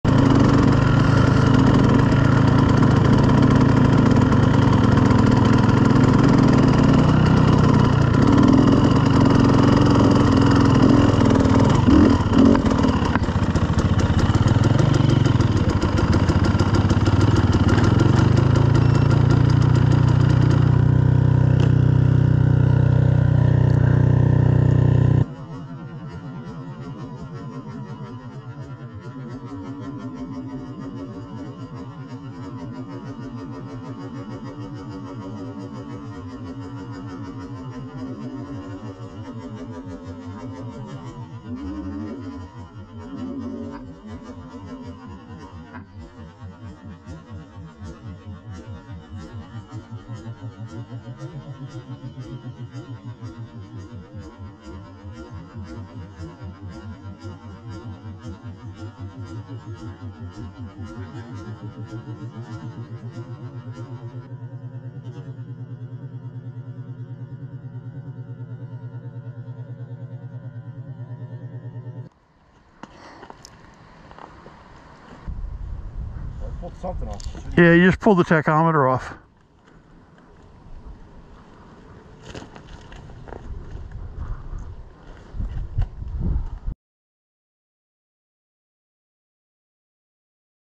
Tom's first drop ever. Burma Road, March 30
Just the drop. Includes super slow-motion version.

Tom was not hurt, not even his pride. It was his first ever ride and the
rocks were big.